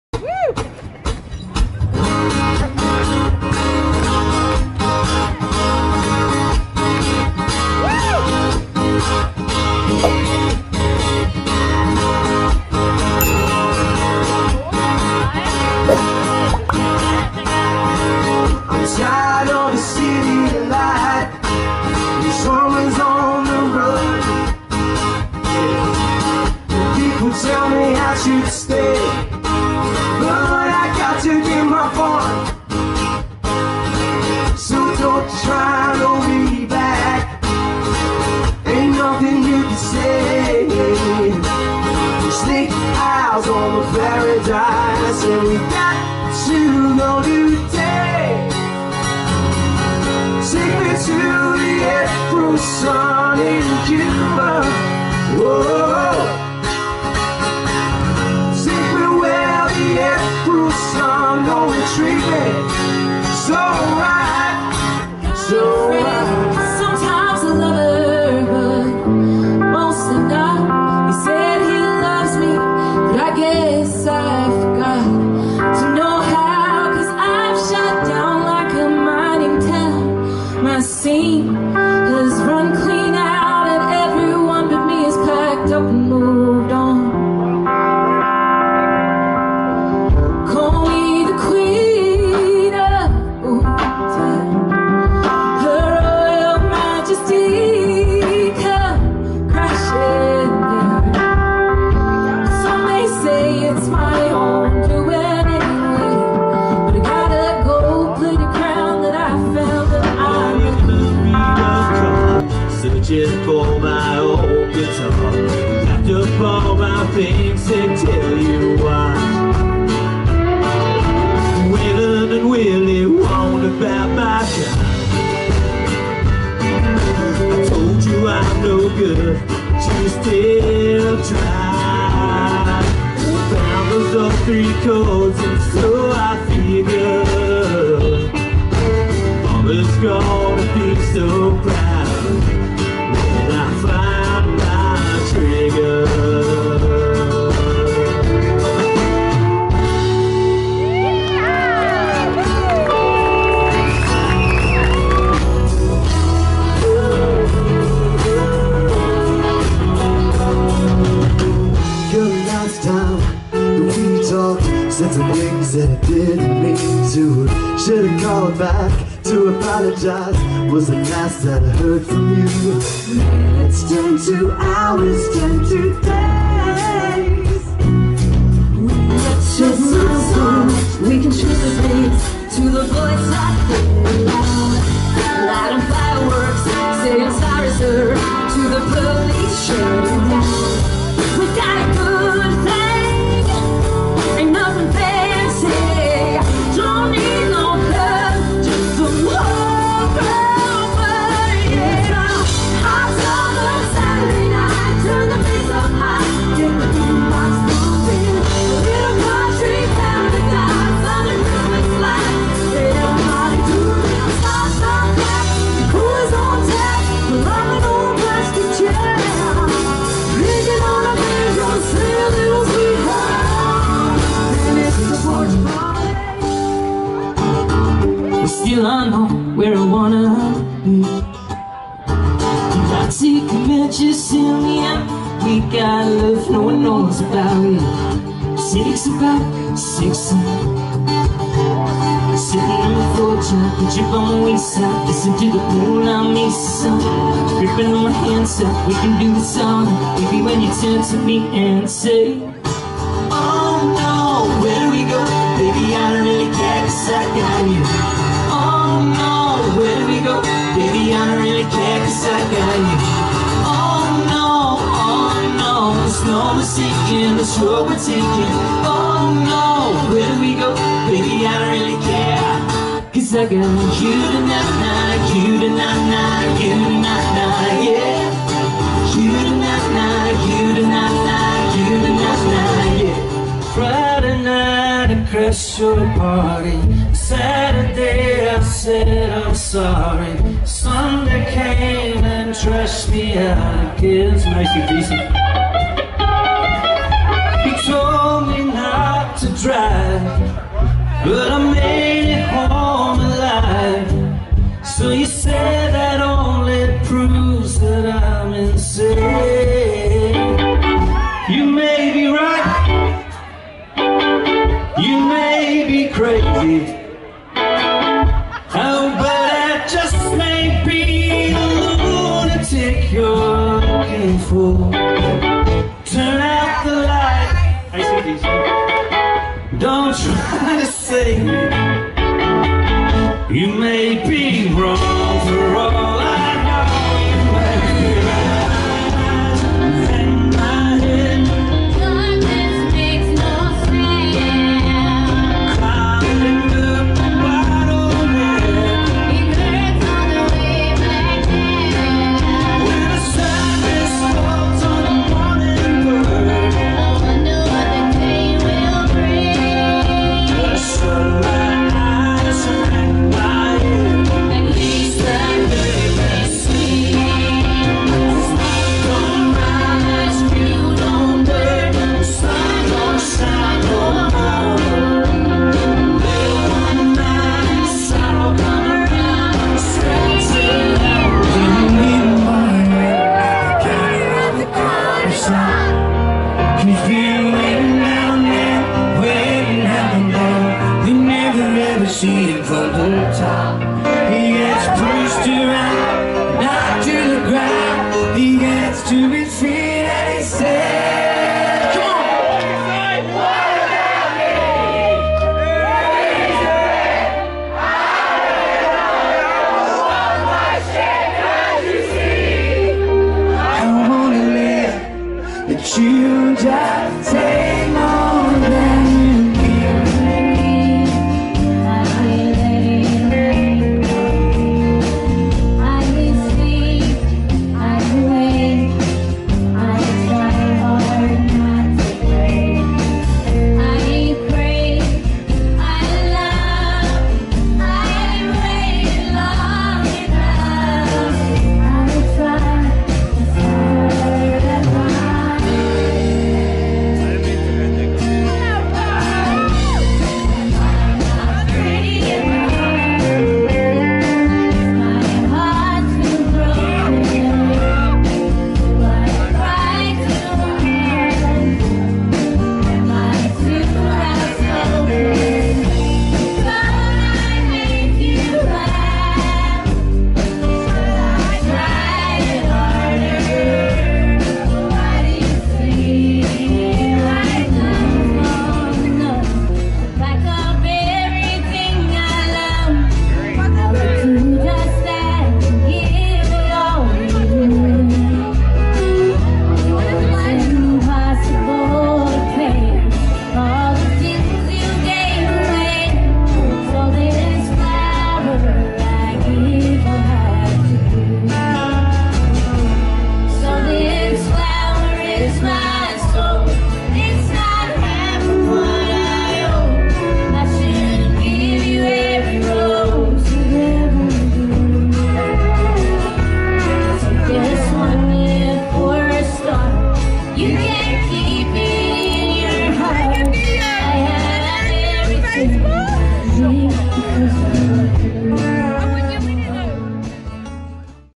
Mornington Country Music Festival 2021
With Bo'Ness - Gretta Ziller - Gareth Leach - Jayne Denham - Darlinghurst Band - Shane Nicholson - Shannon Noll and Kasey Chambers